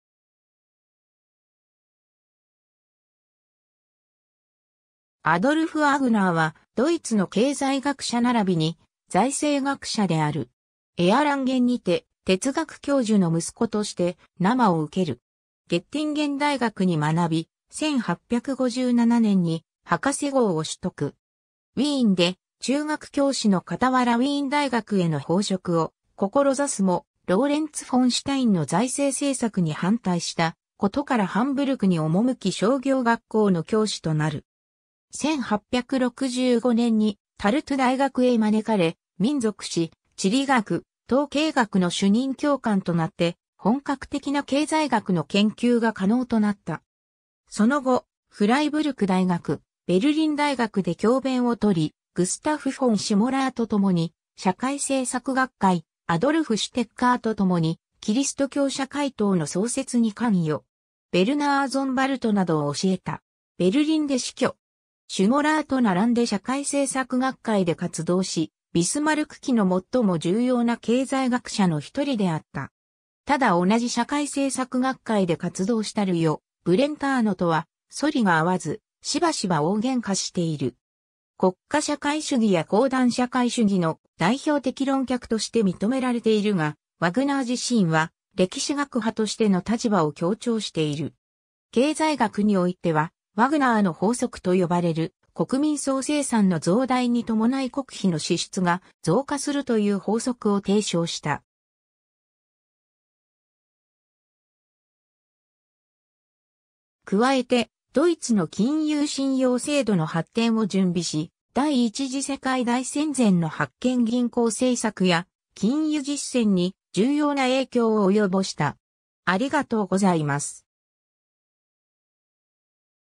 アドルフ・ワーグナー (経済学者)
アルベルト・ルートヴィヒ大学フライブルクの教員
アドルフ・ワグナー（Adolf Heinrich Gotthilf Wagner、1835年3月25日 - 1917年11月8日）は、ドイツの経済学者並びに財政学者である。
 エアランゲンにて哲学教授の息子として生を受ける。
ゲッティンゲン大学に学び、1857年に博士号を取得。
 ウィーンで中学教師の傍らウィーン大学への奉職を志すも、ローレンツ・フォン・シュタインの財政政策に反対したことからハンブルクに赴き商業学校の教師となる。
1865年にタルトゥ大学へ招かれ、民族誌・地理学・統計学の主任教官となって本格的な経済学の研究が可能となった。
 その後、フライブルク大学・ベルリン大学で教鞭を取り、グスタフ・フォン・シュモラーとともに社会政策学会、アドルフ・シュテッカー(de)とともにキリスト教社会党(de)の創設に関与。
ヴェルナー・ゾンバルトなどを教えた。
 ベルリンで死去。
 シュモラーと並んで社会政策学会で活動し、ビスマルク期の最も重要な経済学者の一人であった。
ただ同じ社会政策学会で活動したルヨ・ブレンターノとはそりが合わず、しばしば大喧嘩している。
国家社会主義や講壇社会主義の代表的論客として認められているが、ワグナー自身は歴史学派としての立場を強調している。
 経済学においては、ワグナーの法則と呼ばれる国民総生産の増大に伴い国費の支出が増加するという法則を提唱した。
加えてドイツの金融信用制度の発展を準備し、第一次世界大戦前の発券銀行政策や禁輸実践に重要な影響を及ぼした。